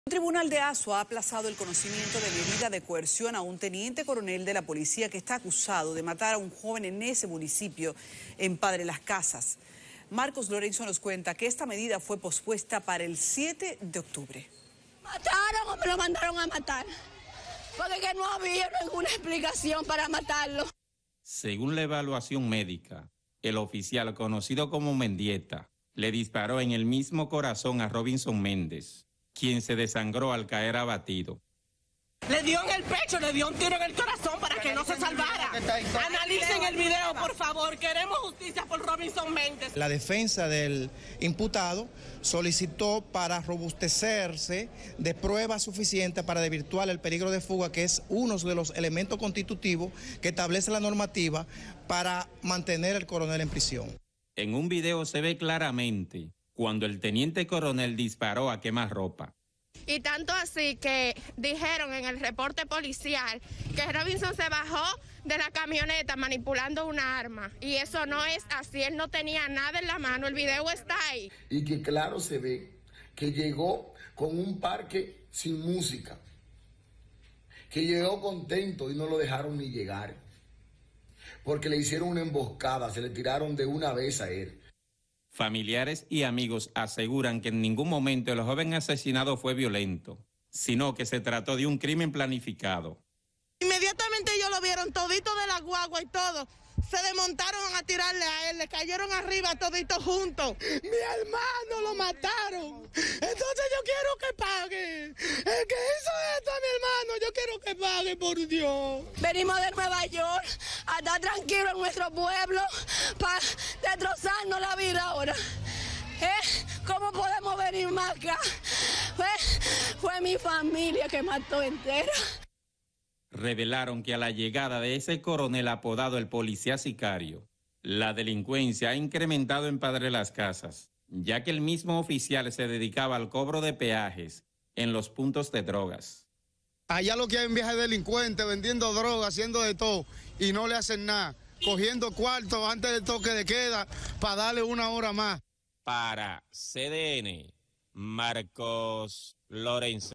Aplazan medida de coerción en caso de teniente coronel PN por muerte de joven en padre las casas
Un tribunal de Azua aplazó el conocimiento de medidas de coerción a un teniente coronel de la Policía acusado de matar a joven en el municipio Padre Las Casas. Marcos Lorenzo nos cuenta que dicha medida fue pospuesta para el día 7 de octubre.